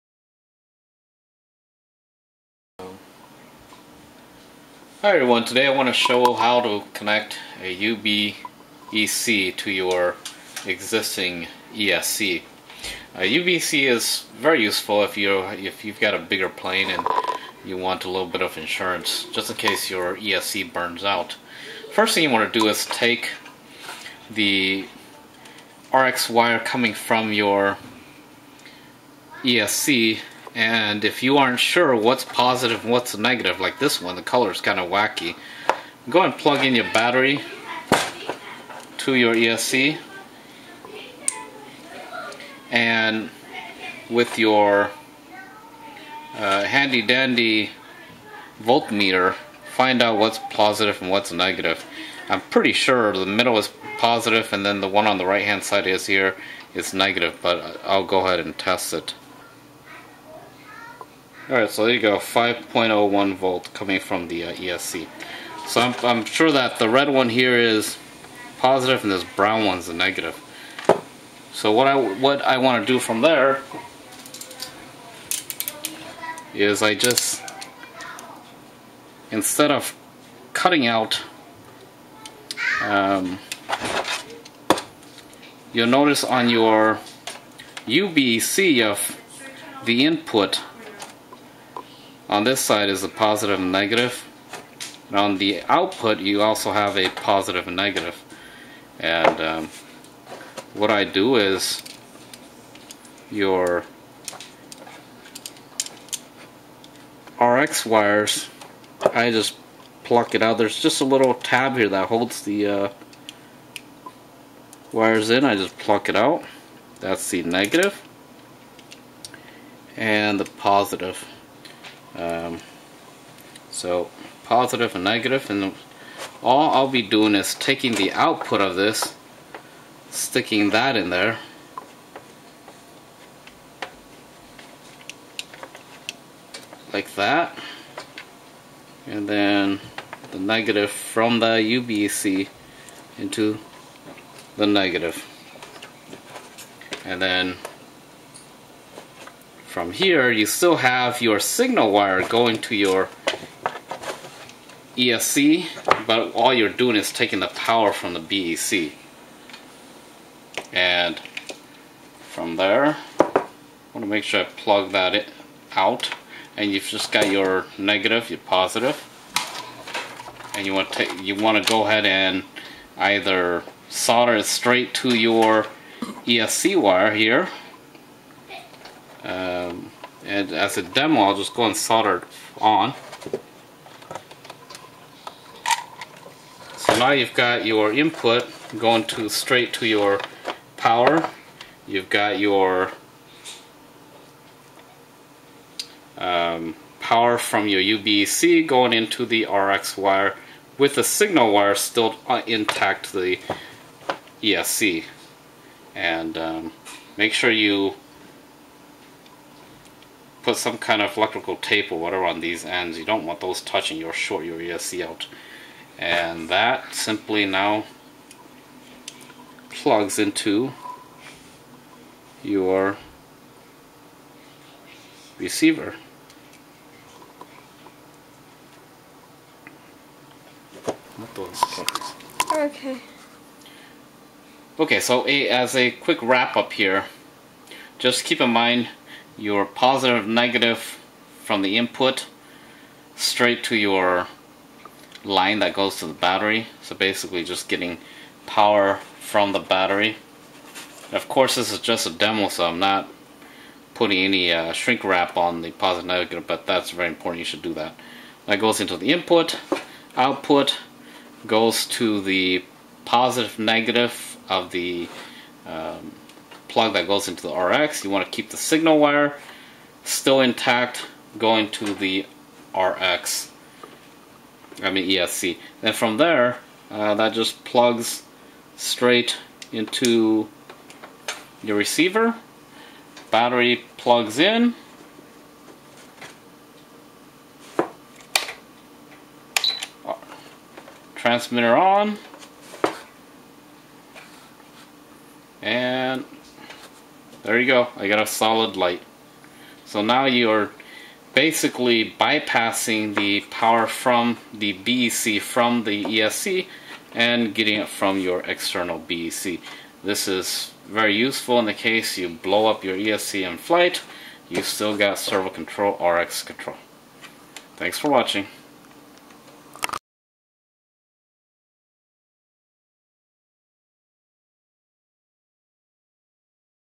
External UBEC "How To"
A fellow youtuber asked me to put this vid together...

This is a quick "how to" on hooking up an external UBEC to your existing ESC, which basically bypasses your internal BEC

Pls keep in mind this is a demo only and I did not take precautionary measures on taping up + and - wires

Another thing to note is make sure you don't swap the + and - wires...doing so will blow you rx.